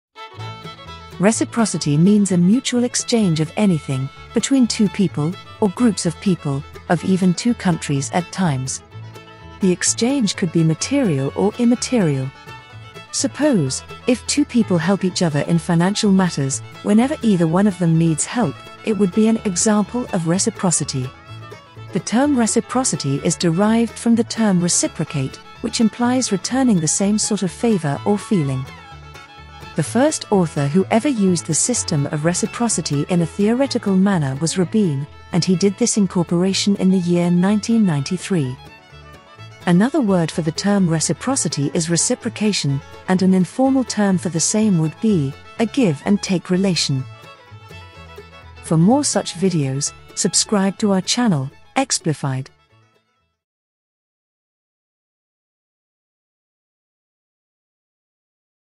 What Is Reciprocity: Reciprocity Meaning Explained
Watch this video know about reciprocity.

Reciprocity means a mutual exchange of anything between two people or groups of people of even two countries at times. The exchange could be material or immaterial.